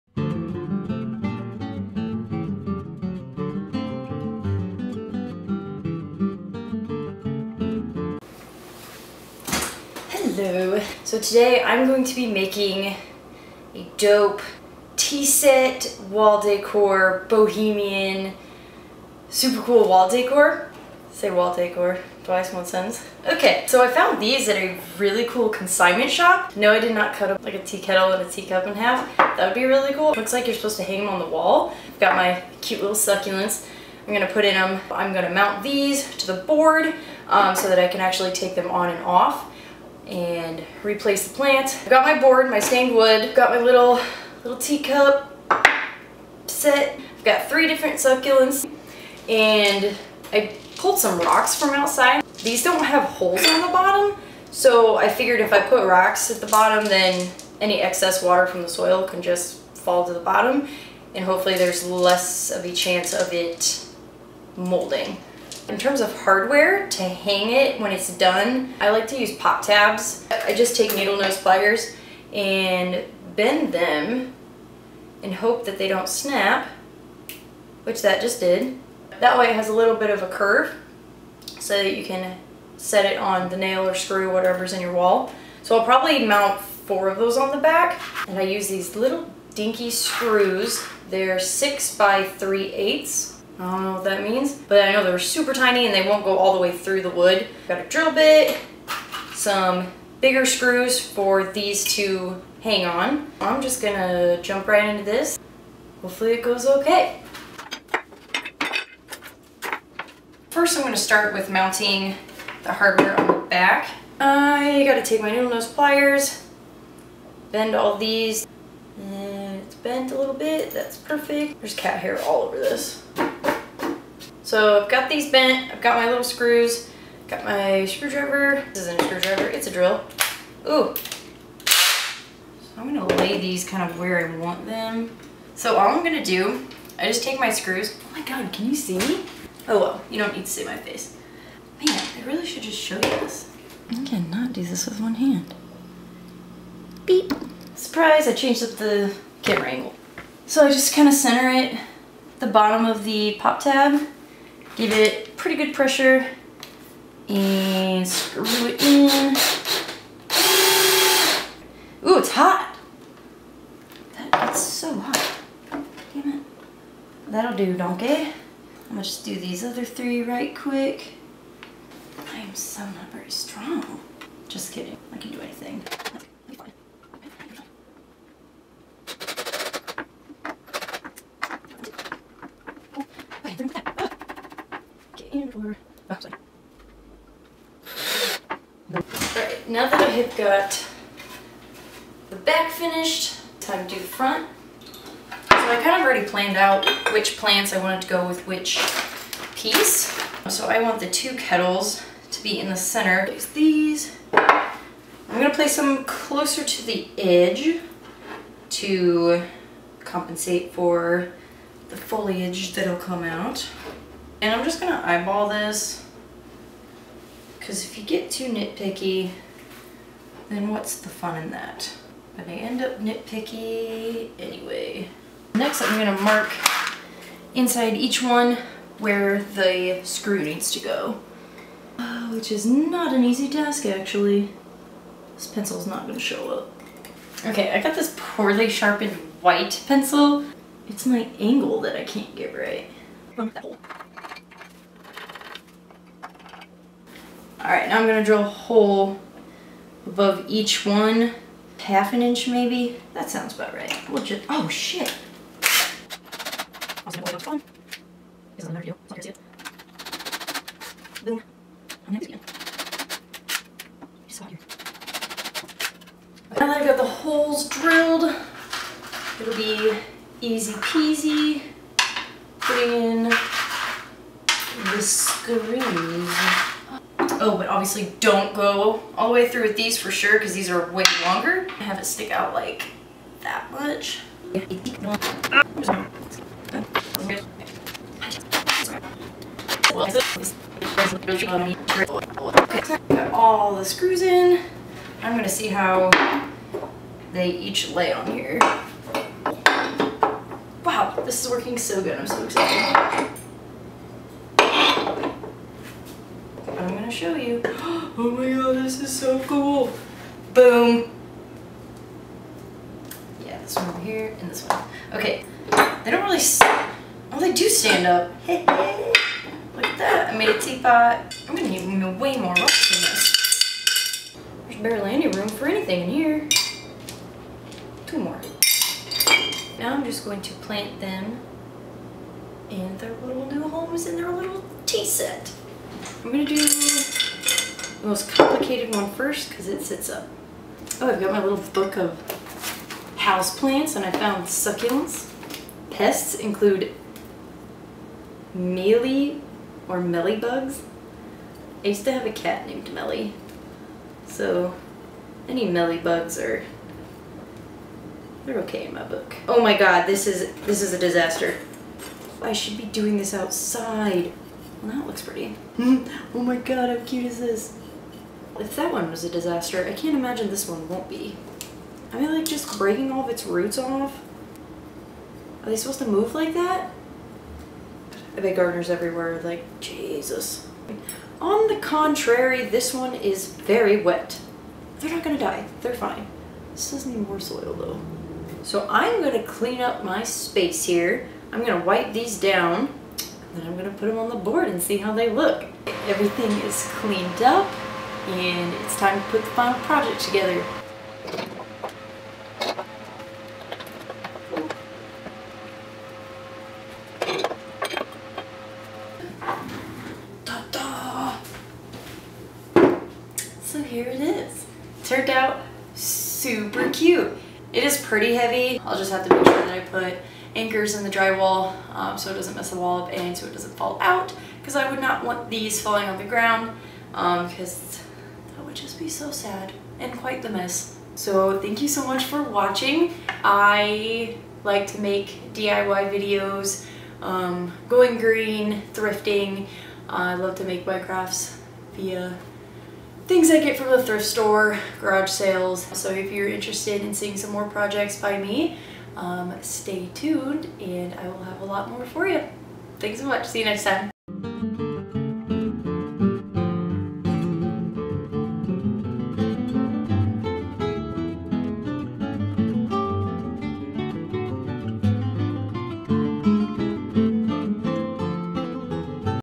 Dysfunctional DIY 01 / Repurposed Tea Set Wall Decor / DIY Succulent Art // Walking Through Flowers
Repurposed Tea Set Wall Decor / DIY Succulent Art

Follow along as I create a beautiful handmade piece of eco friendly wall art with a thrifted tea cup and pot wall set, a piece of stained wood, and succulents.